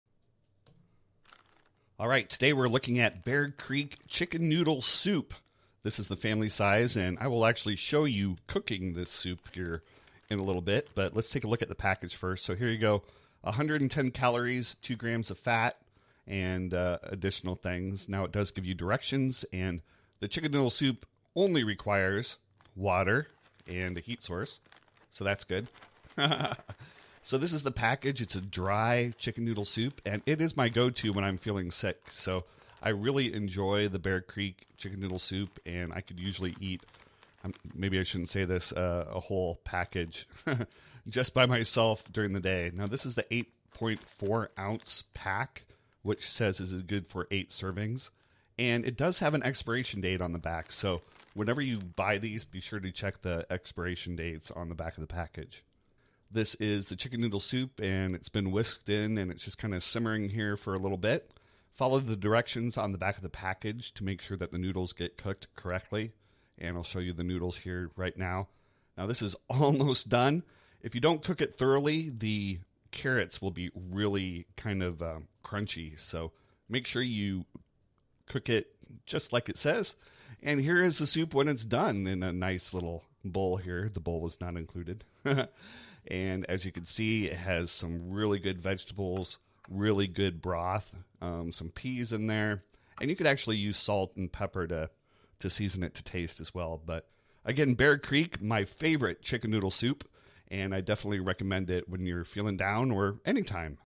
Best chicken noodle soup: Bear Creek Chicken Soup Mix
This is my favorite dry chicken soup mix. It is so good - sometimes I eat it all at one sitting!